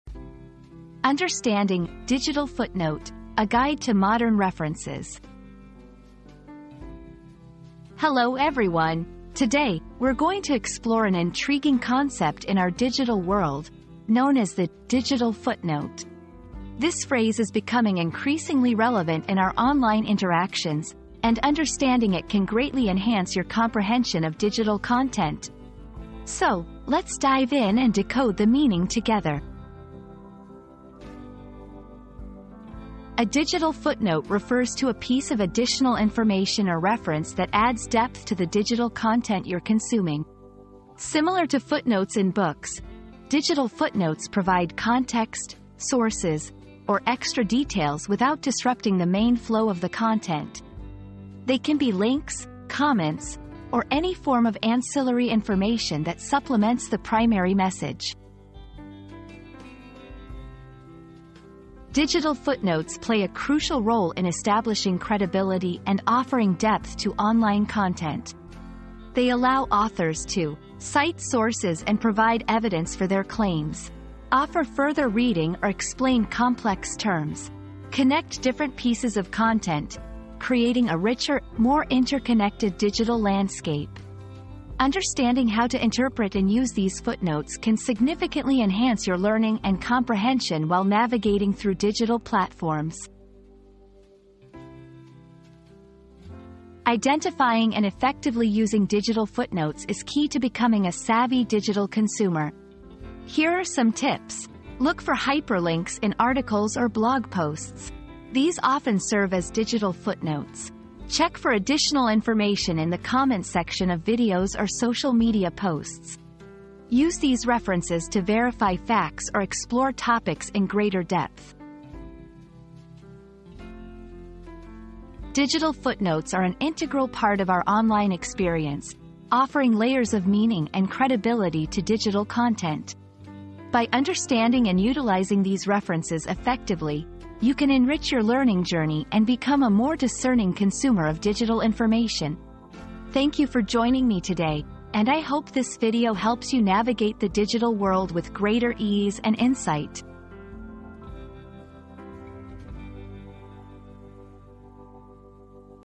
Understanding "Digital Footnote": A Guide to Modern References
Unraveling the Digital Footnote: A Modern Guide to References • Discover the secrets of the digital footnote in this engaging guide. Learn how to navigate modern references and enhance your research skills in the digital age.

00:00 • Introduction - Understanding "Digital Footnote": A Guide to Modern References
00:32 • What is a Digital Footnote?
01:01 • The Role of Digital Footnotes in Online Content
01:36 • How to Identify and Use Digital Footnotes